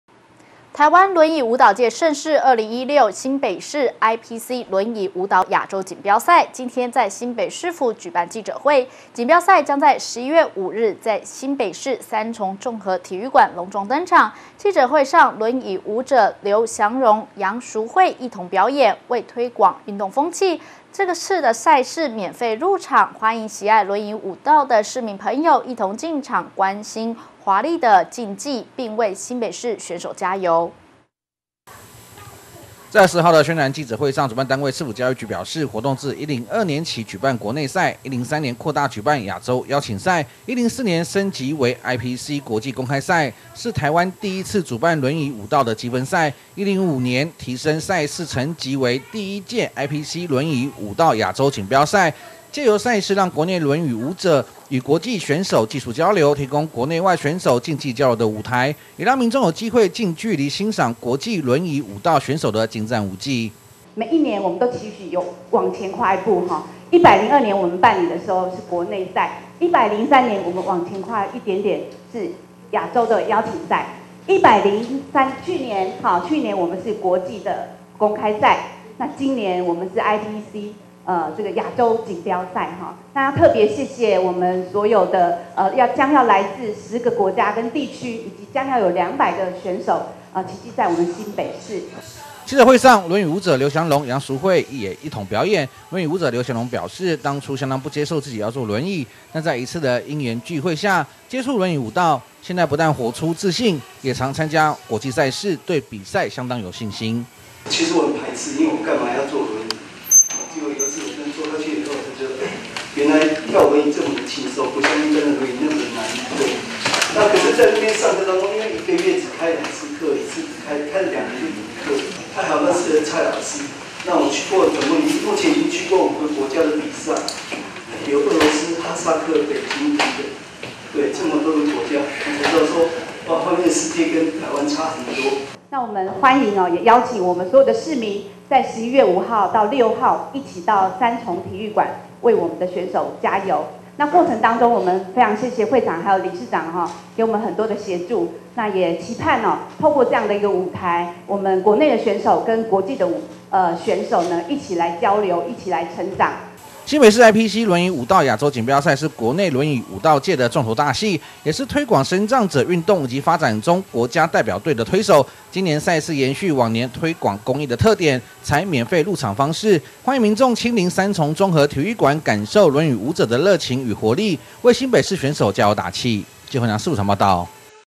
1051020觀天下新聞HD02 新北市輪椅舞蹈亞洲賽 11月5日華麗登場
臺灣輪椅舞蹈界盛事「2016新北市IPC輪椅舞蹈亞洲錦標賽」今（20）日在新北市政府舉辦宣傳記者會，錦標賽將於11月5日在新北市三重綜合體育館隆重登場，記者會上輪椅舞者劉祥榮、楊淑惠一同表演。為推廣運動風氣，本賽事免費入場，歡迎喜愛輪椅舞蹈的市民朋友一同進場觀賞華麗的競技，並為新北市選手加油。